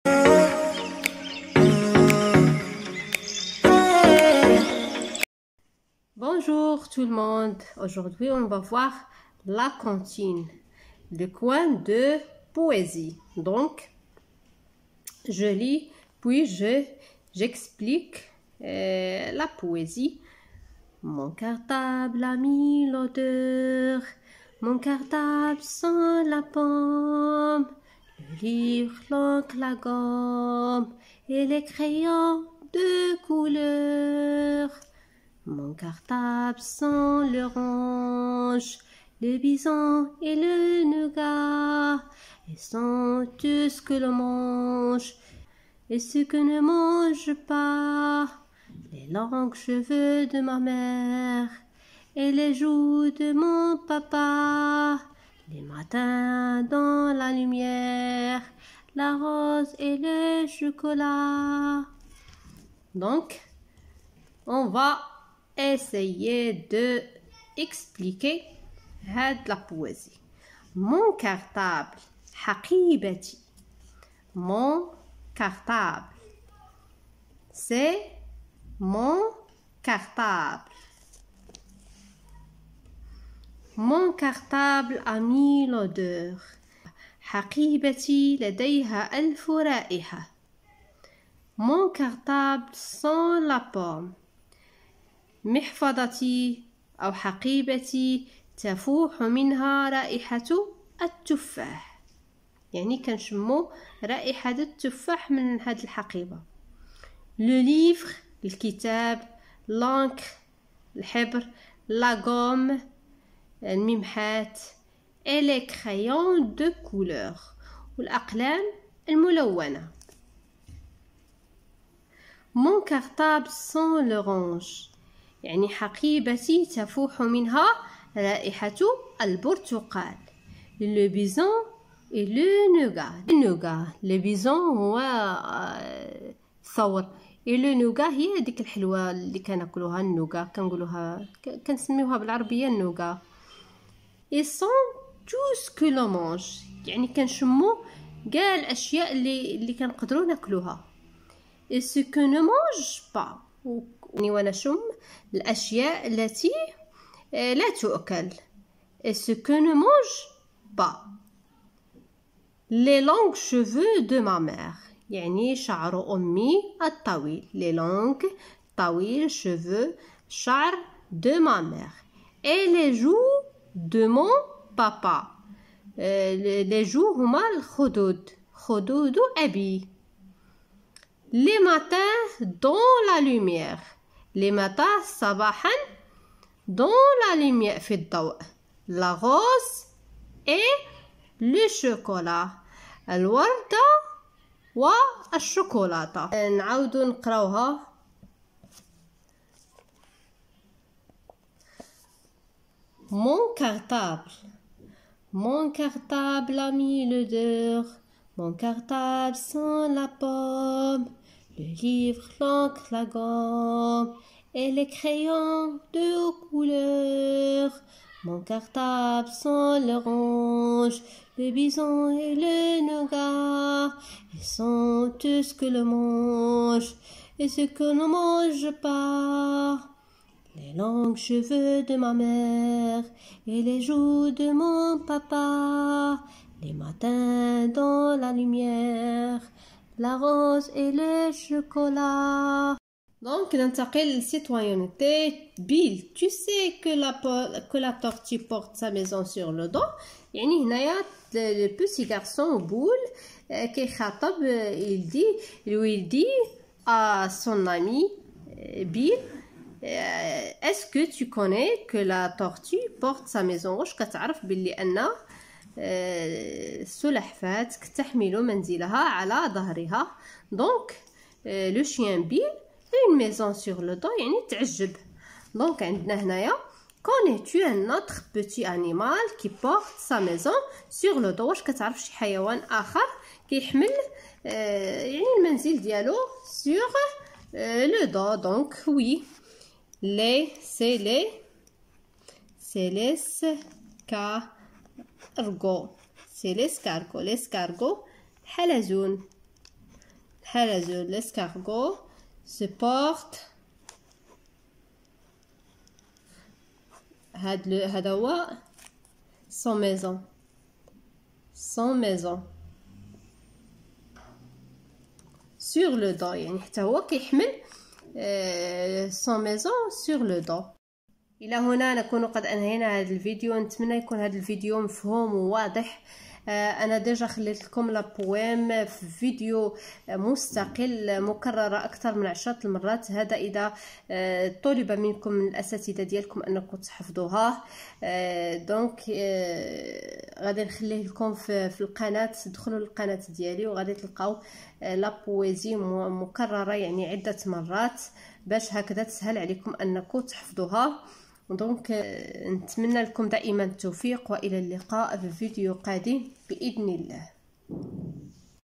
l'oasis des mots français 3ème année primaire page :19 /la comptine :mon cartable /explication
Poésie : Mon cartable
La comptine : Mon cartable
Mon cartable a mille odeurs
Je lis et je comprends
L'oasis des mots page 19
Mon cartable
L'oasis des mots français 3ème page 19
Répétitions :